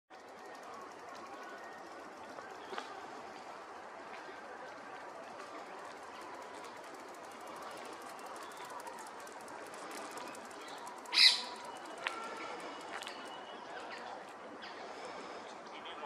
Black rumped flameback woodpecker on a foggy winter morning at Bharatpur National Park
Species-Black rumped flameback woodpecker
Location-Bharatpur bird sanctuary, Rajasthan, India
Date-December 2022